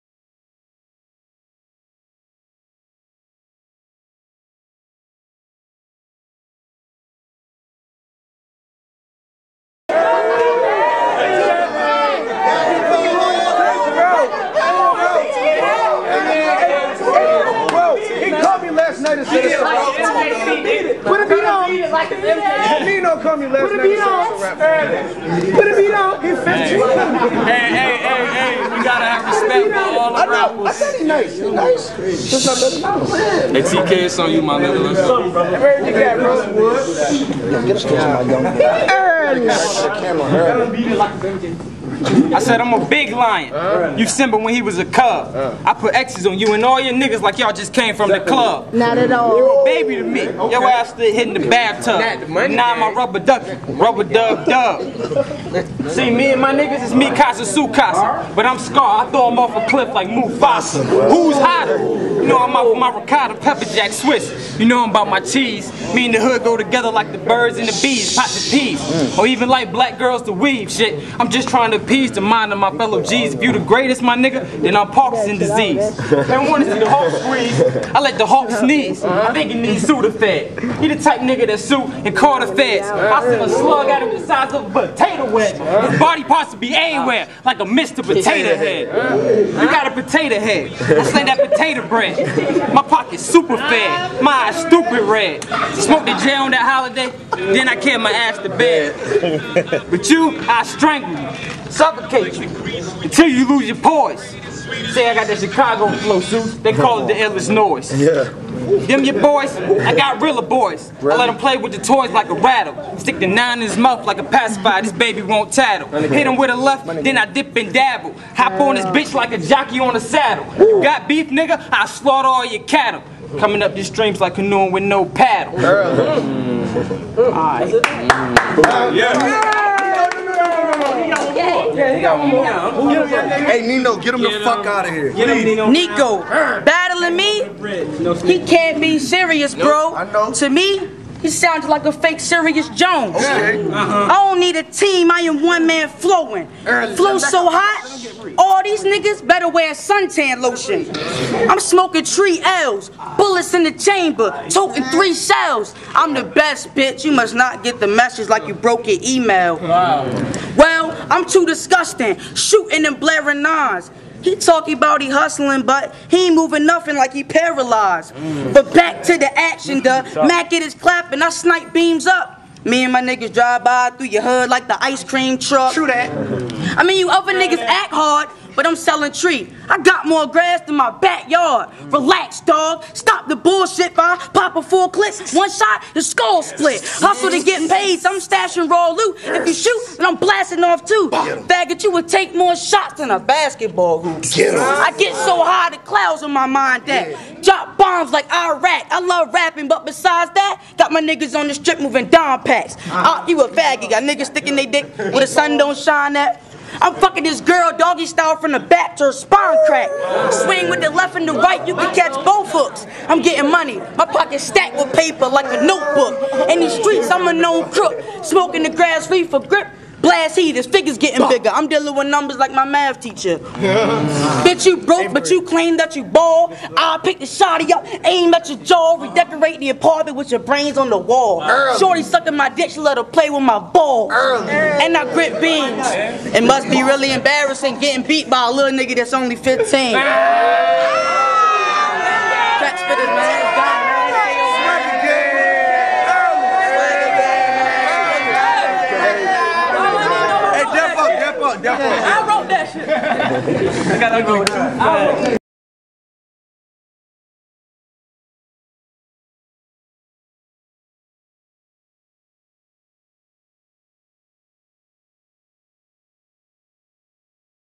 Nino Vs TK final round
Nino Vs TK Final round at Head Hunters in Norfolk,Va